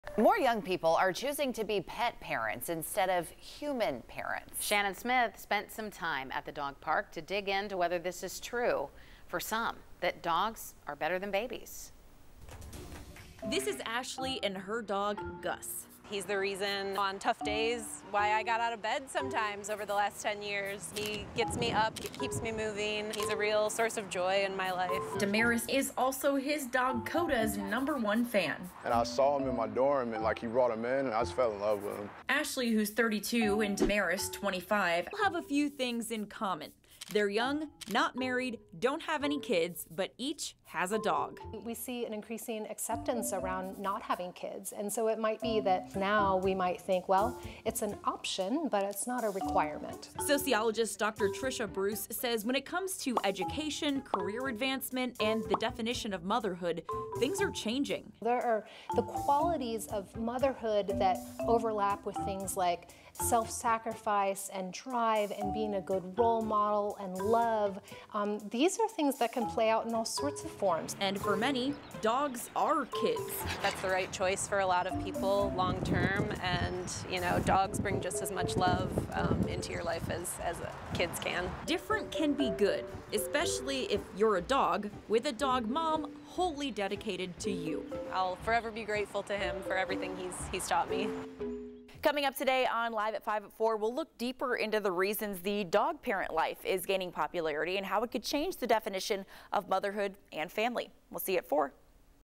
Diapers or Dog Treats? Why more young people are choosing to be pet parents
Birth rates are down, pet adoptions are up and women who feel called to be mothers don't necessarily want to be mothers to children. We went to the dog park to find out why.